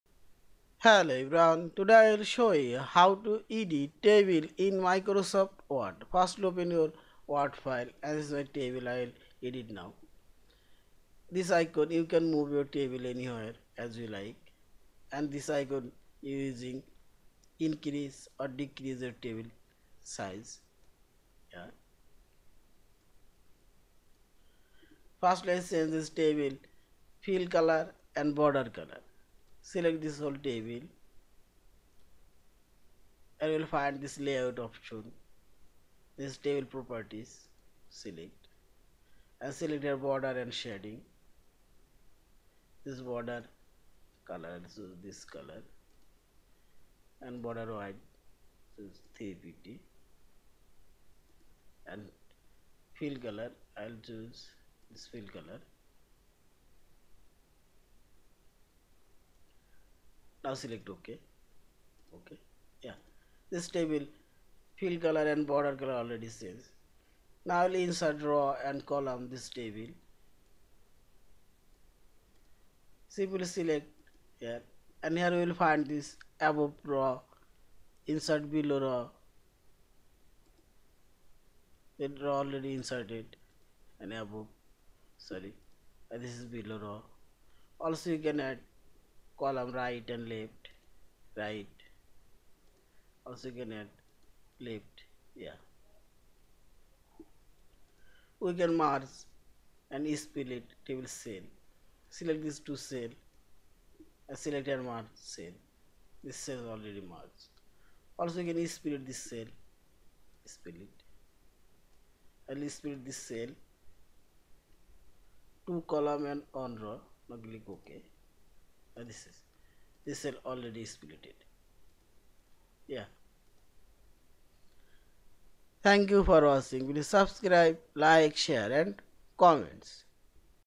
How to edit or change a table in Microsoft Word Document
Hello everyone, Today I will show you, How to edit or change a table in Microsoft Word Document.